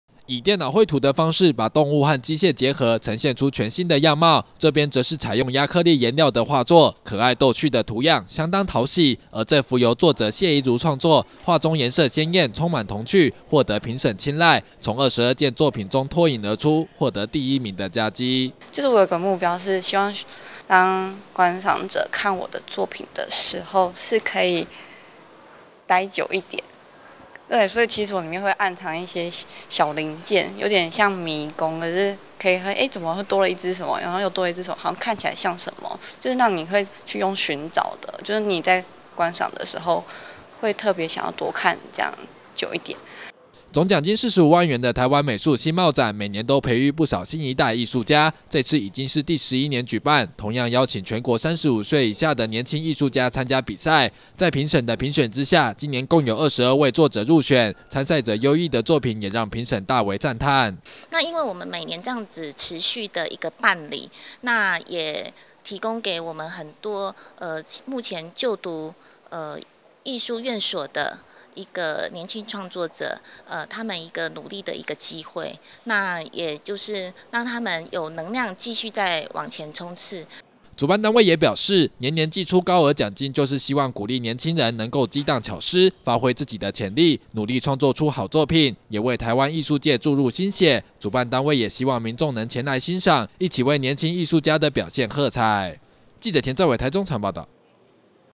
大台中新聞-清水台灣新貌展1205
獎金45萬元的台灣美術新貌展，已經連續舉辦第11個年頭，希望廣邀全國35歲以下的年輕藝術家參加比賽，為藝術注入新血，而今年共入選22位作者，每人提供三幅以上的作品，主辦單位表示，希望藉由比賽，鼓勵更多年輕藝術家投入創作，並藉由展出讓民眾了解台灣藝術的發展與變化。